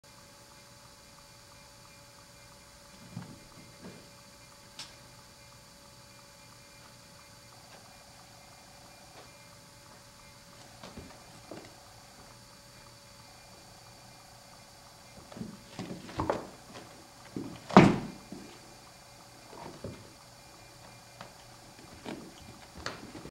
Ben the Bengal
Bengal kitten performs somersaults